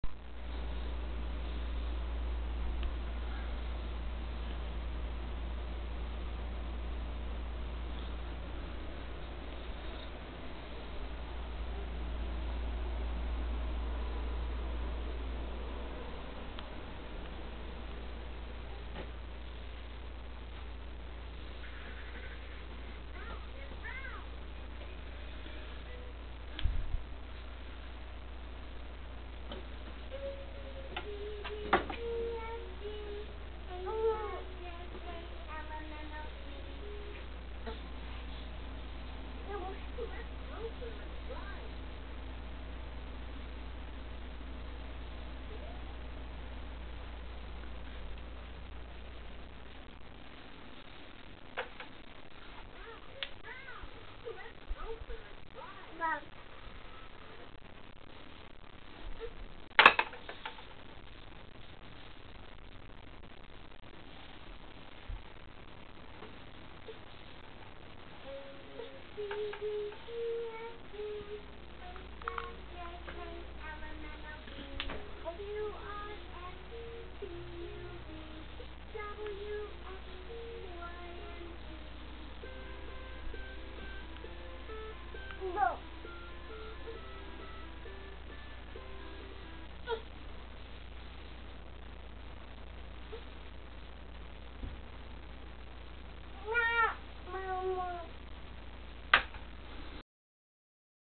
Evie and the Giant Bunny (13 Months)
It's nearly as big as she is.